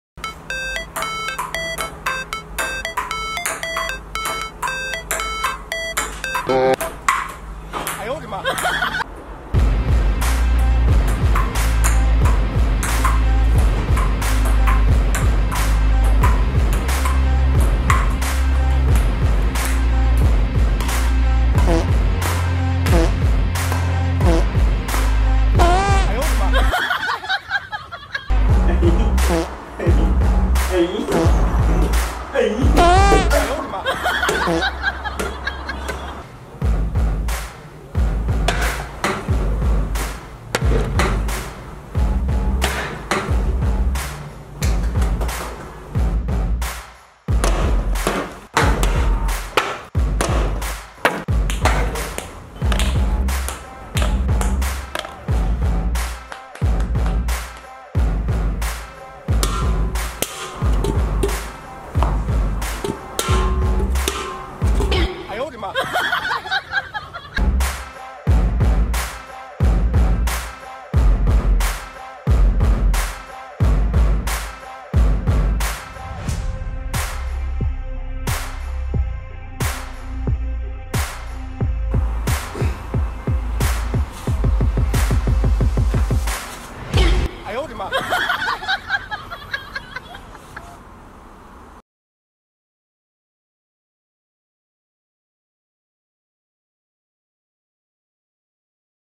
Can Dog Plays Volleyball?! 😲 | TikTok Funny Animal | Funny and Cute Pets Video | SHIBA Duoduo's Life
Talented dog Duoduo plays table tennis, badminton, volleyball with his owner.
He works hard before the game to win ! ! !
What else can I say? It's amazing! 🤣🤣🤣👍👍👍
Funny Dog Videos 2022
Enjoy my weekly funny videos ! ! !
and
                          from Duoduo
                                   A talented Shiba Inu